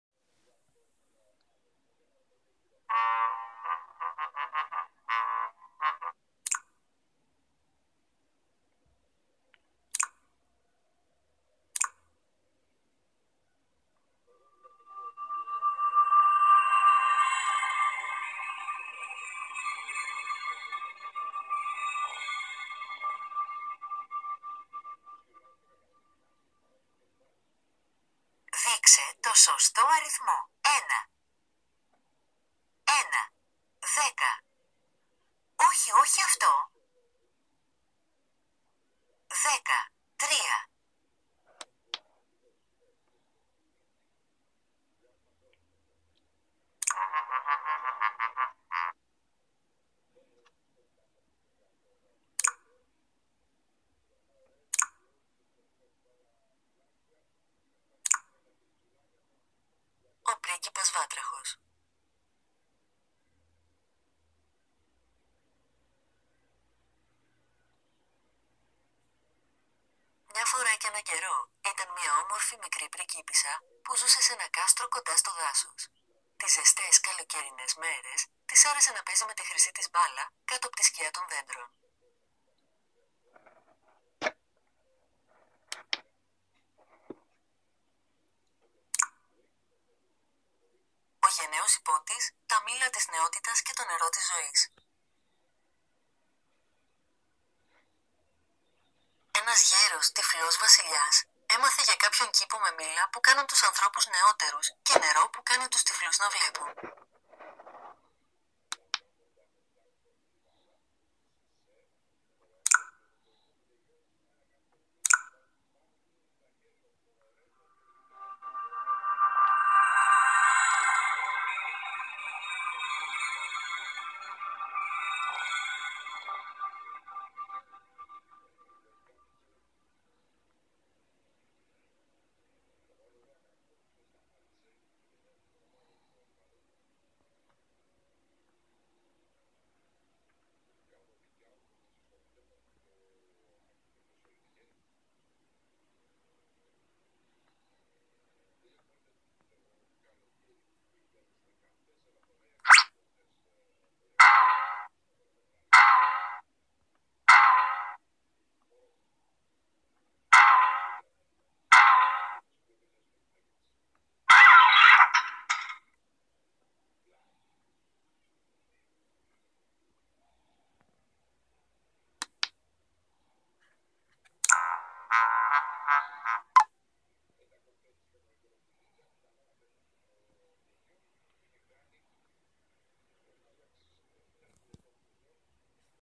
MLS iQtab kido Extra
Short video of the latest tablet for children from Greek company MLS